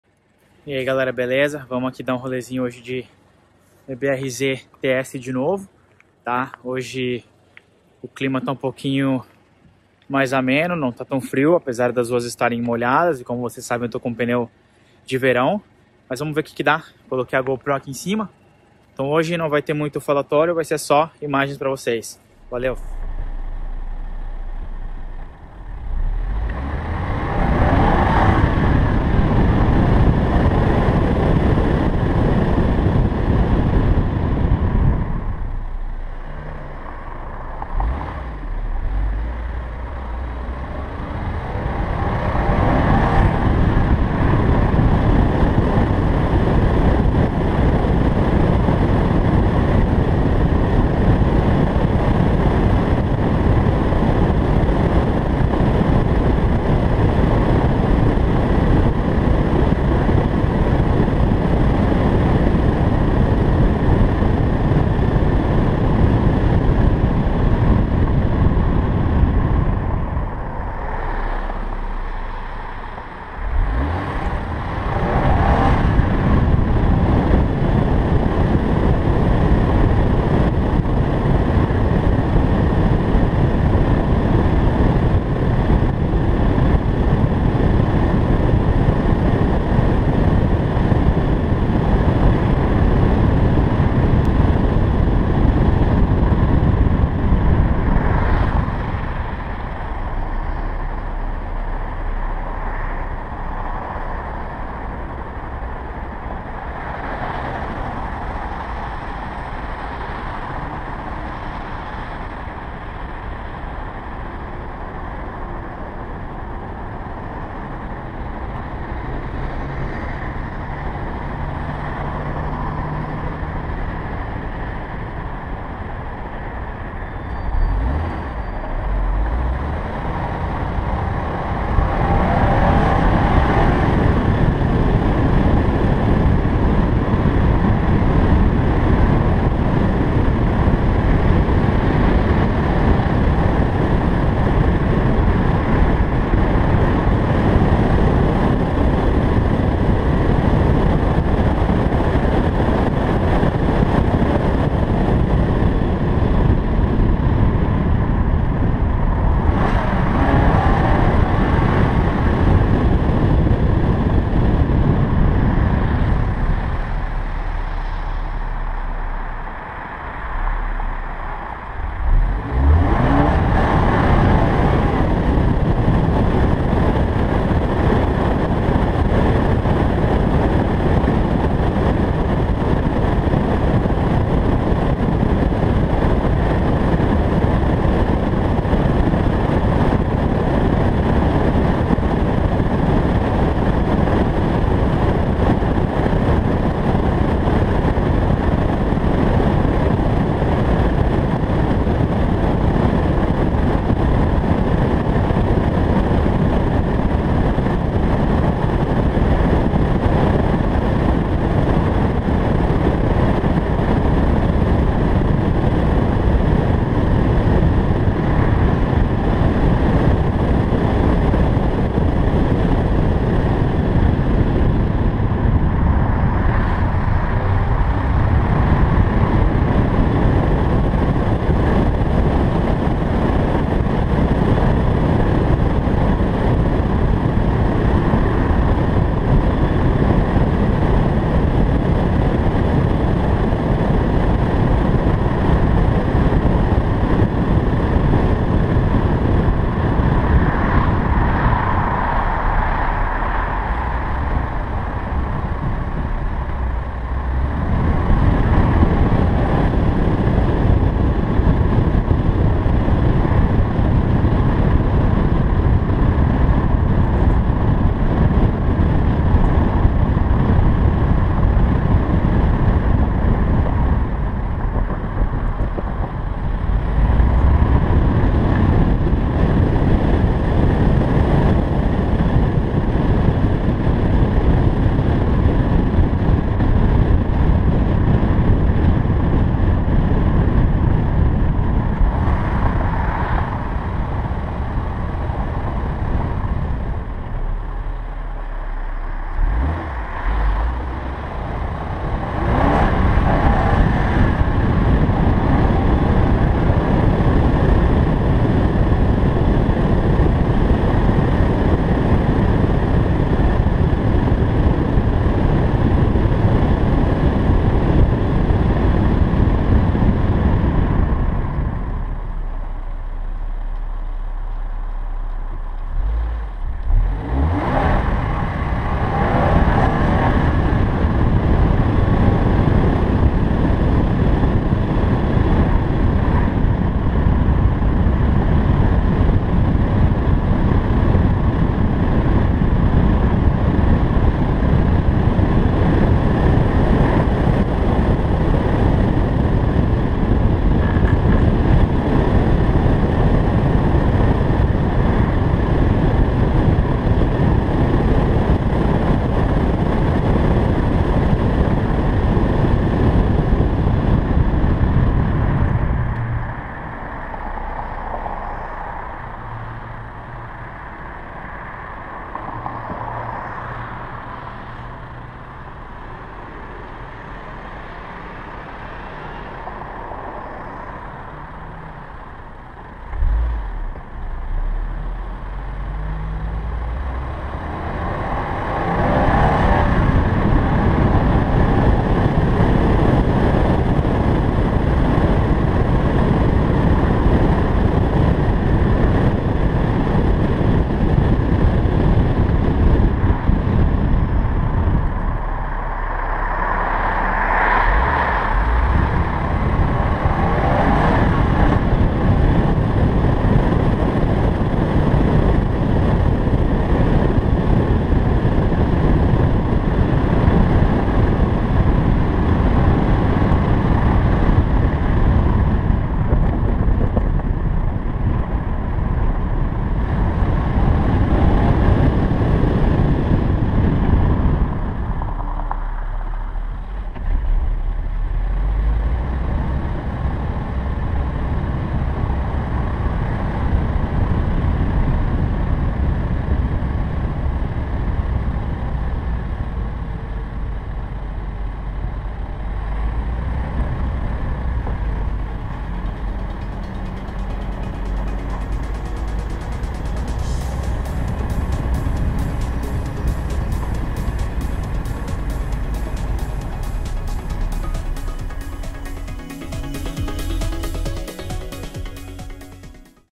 Rolê de BRZê
PT-BR: Rolê de BRZ tS, testando os pneus de verão no inverno.
EN-CA: Testing my summer tires during the mild winter in Ontario.